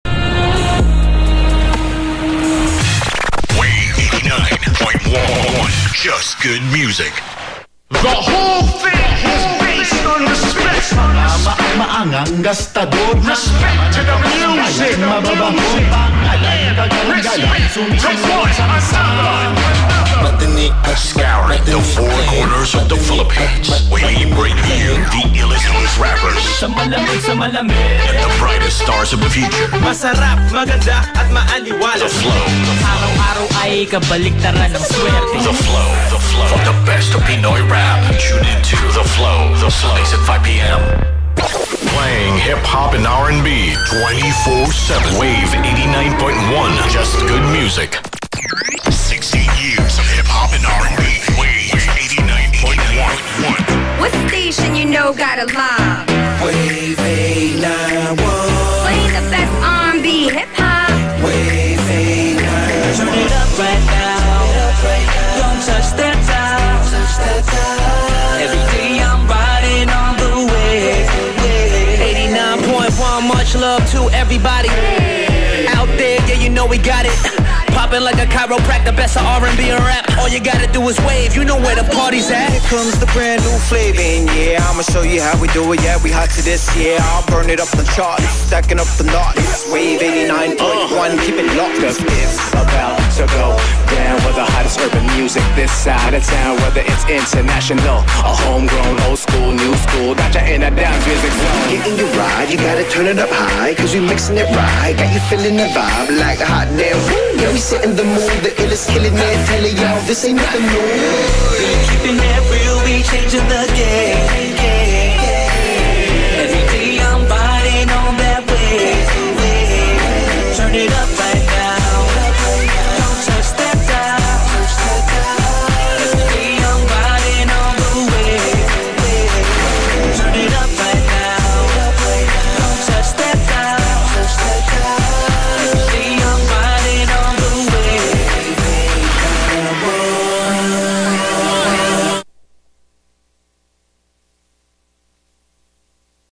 (DWAV-FM) WAVE 89.1 SIGN-OFF
DWAV (89.1 FM) - broadcasting as WAVE 89.1 - is a 24-hour Rhythmic Contemporary radio station that plays a Contemporary R&B, Funk, Motown, Hip-Hop, Old School Rap, Quiet Storm format. It is owned and operated by Blockbuster Broadcasting System, Inc. in the Philippines. The station's studio and transmitter is located at the room 201 Strata 2000 Bldg, Ortigas Center, Pasig City
©Tiger 22 Media Corporation
(Blockbuster Broadcasting System)
No one reads this far down into the description... what are you doing snooping around?